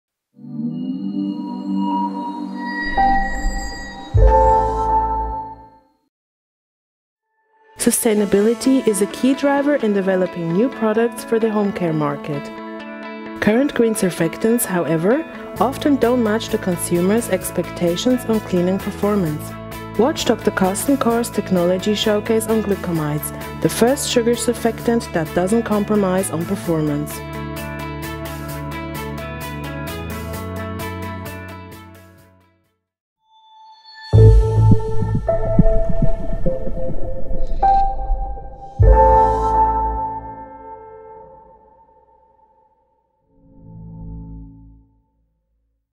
Clariant's Technology Showcase Teaser
Watch a sneak preview of Clariant's Technology Showcase at the AOCS 2014 in Montreux, Switzerland.